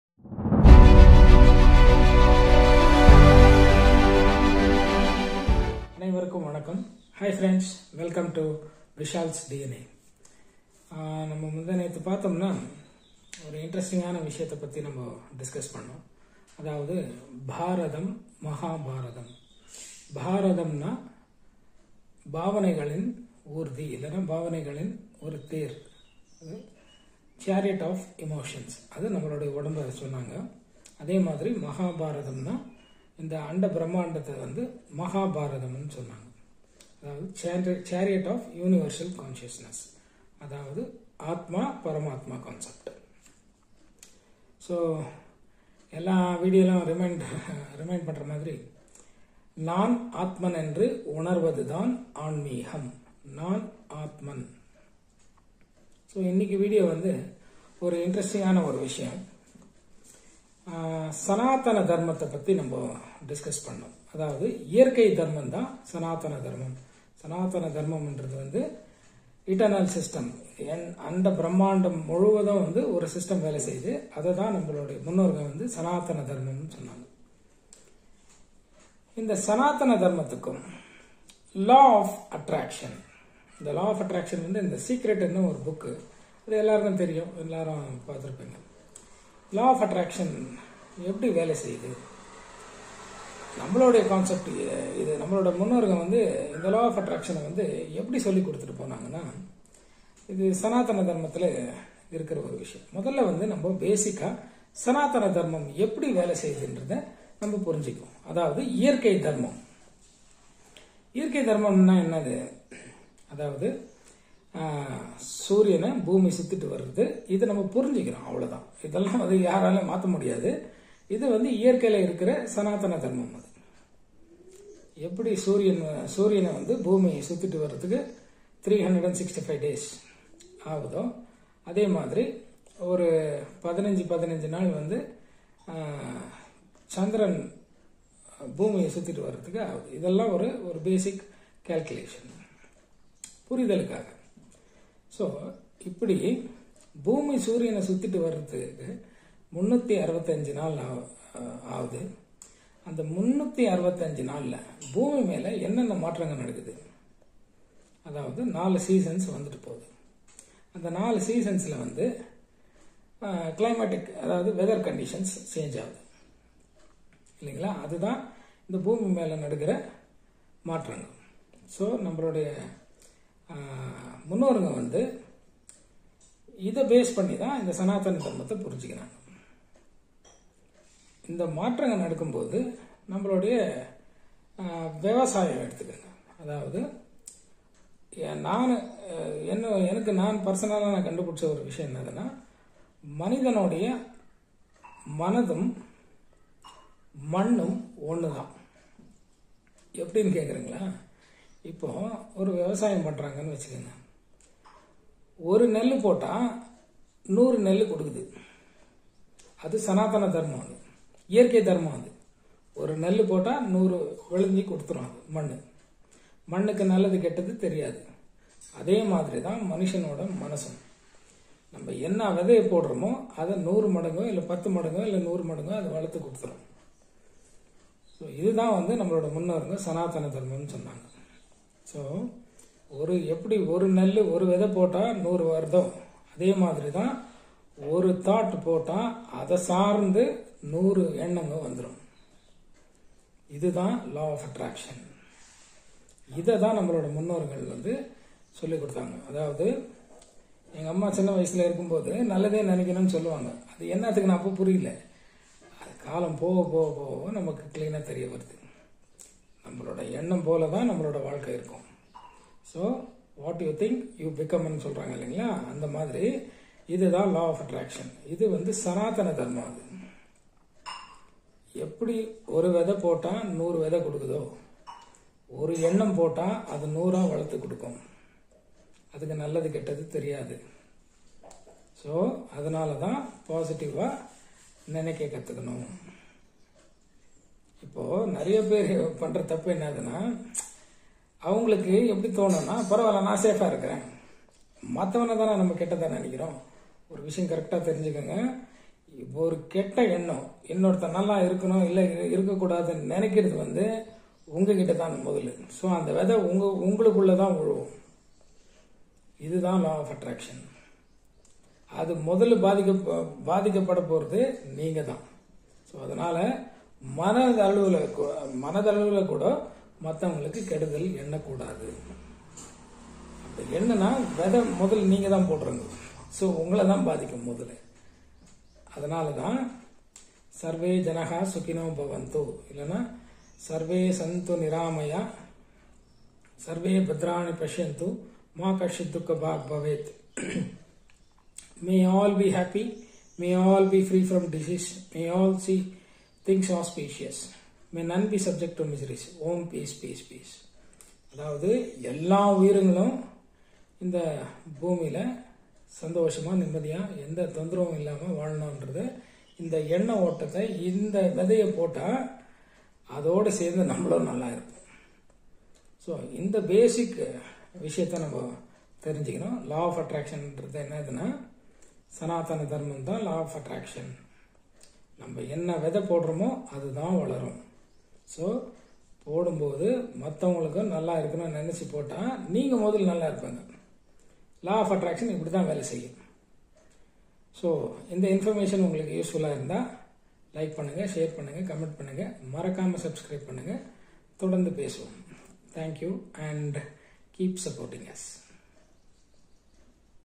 Mastering the Law of Attraction: Manifest Your Desires | Vishal's DNA | VD
Law of Attraction Mastery

"Mastering the Law of Attraction: Manifest Your Desires"

The Law of Attraction is a powerful principle that states that we attract into our lives whatever we focus on, whether it is positive or negative. It is based on the idea that we are all made up of energy, and that this energy vibrates at different frequencies. By aligning our thoughts, feelings, and actions with our desired outcomes, we can attract those things into our lives.

The Law of Attraction has been used by millions of people around the world to manifest their dreams and achieve their goals. It is often associated with the idea of visualization and positive thinking, but it goes much deeper than that. It requires a deep understanding of ourselves, our beliefs, and our intentions.

By learning how to harness the power of the Law of Attraction, we can create a life that is full of abundance, happiness, and success. It takes practice and dedication, but with the right mindset and tools, we can achieve anything we set our minds to.